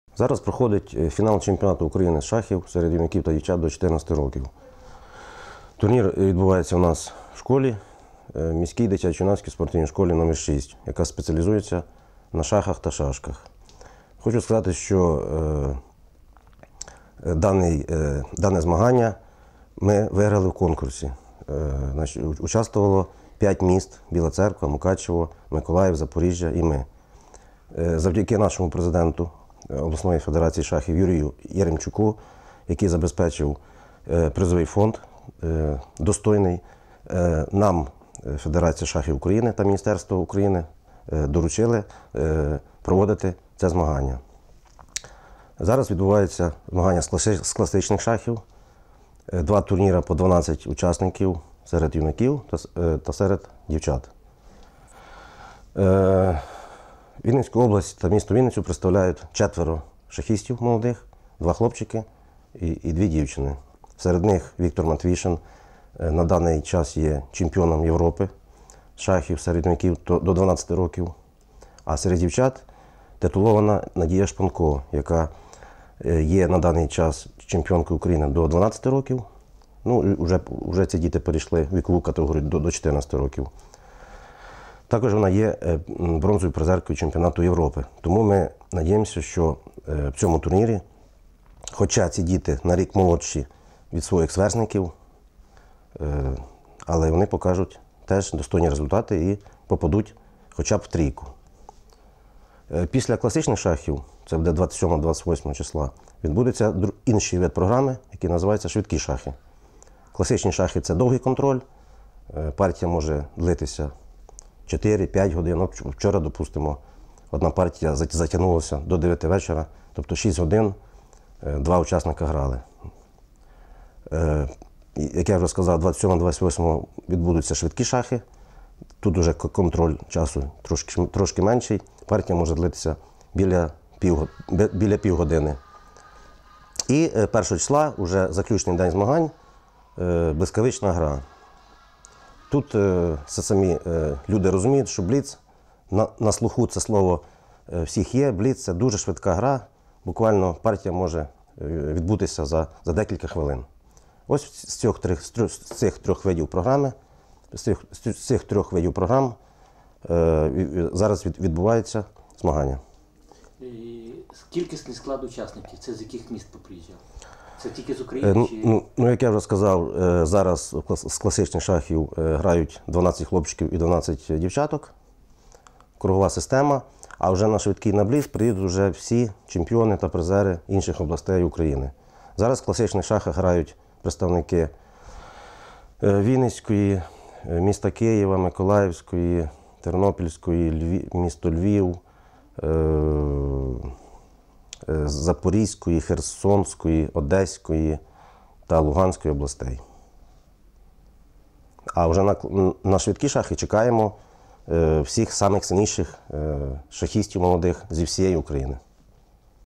Фінал чемпіонату України з шахів
Фінал чемпіонату України з шачів серед юнаків та дівчат до 14 років